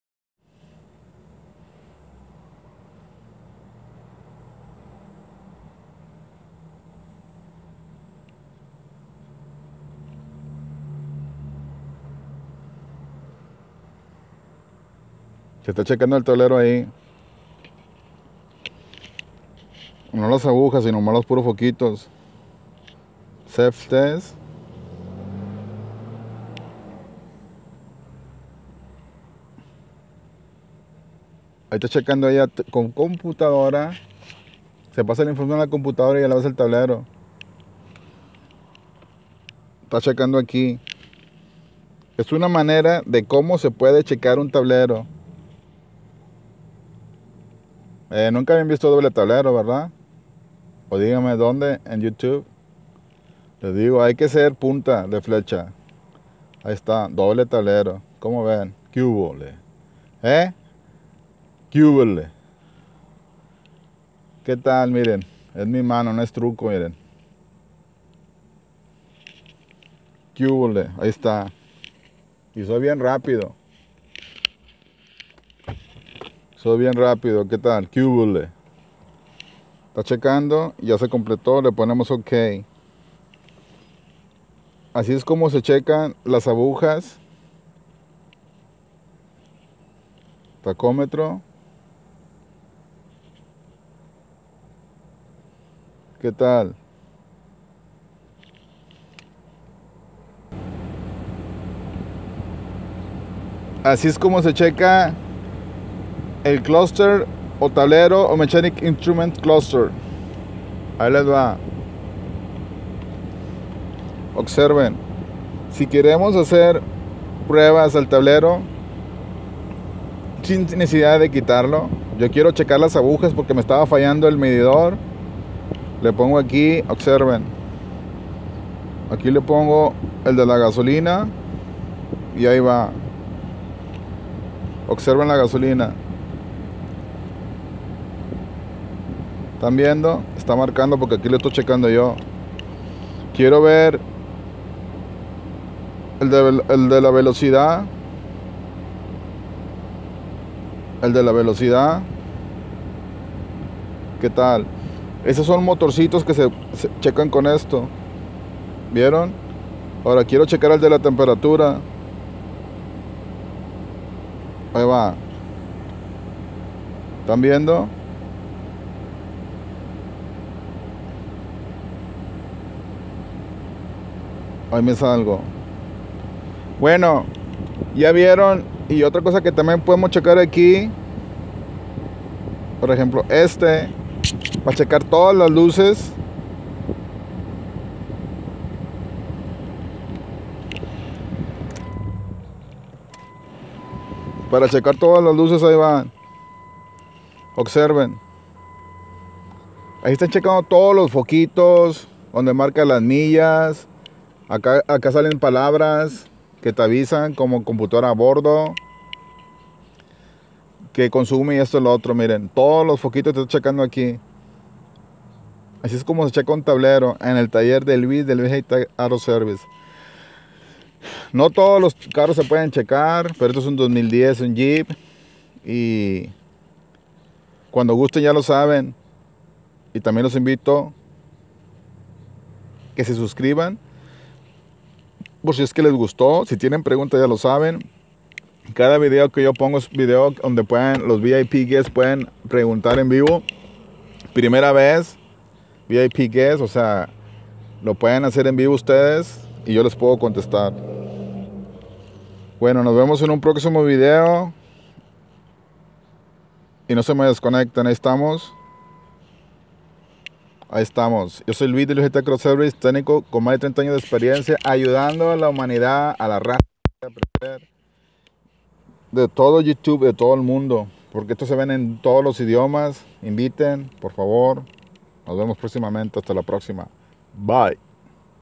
Problema Eléctrico falla del Tablero, IPC, cluster, como checarlo
Tablero 2010 Jeep

les comparto videos de tableros como checar
quieres saber mas  PREGUNTAME  es gratis aqui MISMO
gratis.  video chat gratis te espero!

si no marca  gasolina el A/C  funciona?
porque falla los tableros?